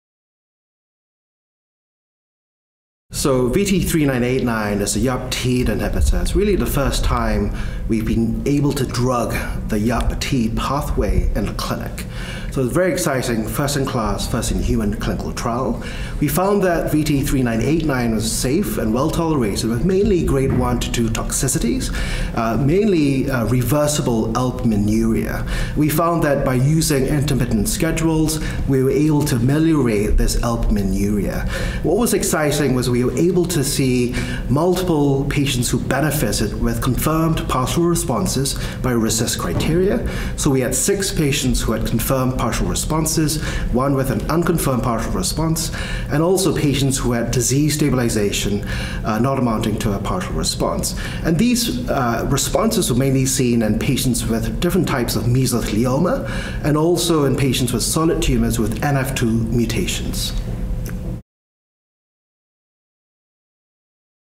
VT3989YAP, a YAP/TEAD inhibitor, in mesothelioma and NF2-mutant cancers
Timothy Yap, MBBS, PhD, FRCP, The University of Texas MD Anderson Cancer Center, Houston, TX, provides an overview of the first-in-call, first-in-human Phase I trial (NCT04665206) of VT3989, a YAP/TEAD inhibitor, in patients with advanced solid tumors enriched for malignant mesothelioma and other tumors with NRF2 mutations. The co-primary endpoints are occurrence of dose limiting toxicity and occurrence of general toxicity. Secondary endpoints include tumor response and pharmacokinetic evaluation. VT3989 was found to be safe and well tolerated and demonstrated antitumor activity in advanced mesothelioma and NF2-mutant cancers. This interview took place at the American Association for Cancer Research (AACR) Annual Meeting 2023 in Orlando, FL.